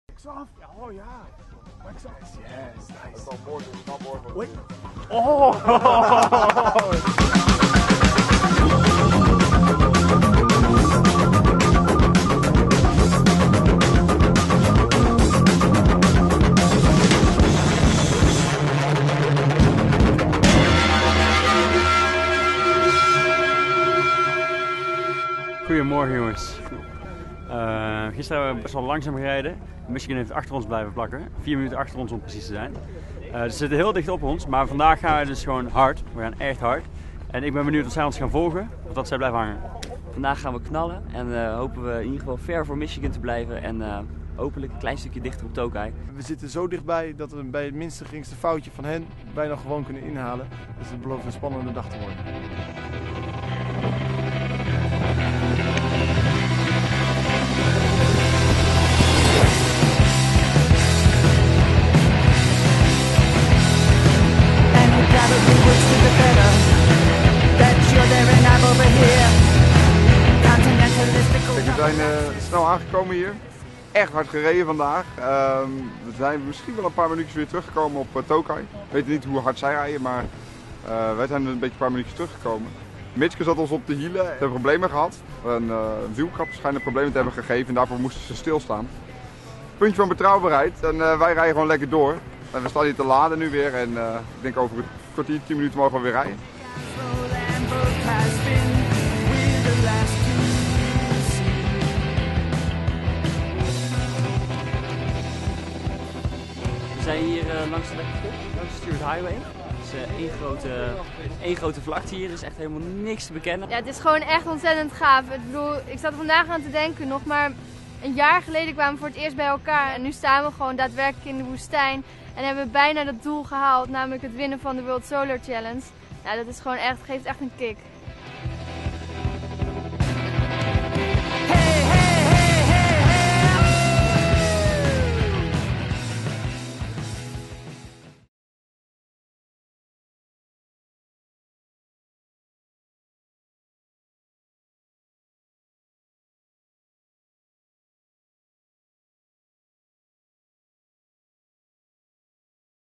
Day 4 - Chasing The Rising Sun
Having passed all other cars by far Nuna6 is still chasing Tokai, with Michigan still hot on its tail.
"Wax on, wax off"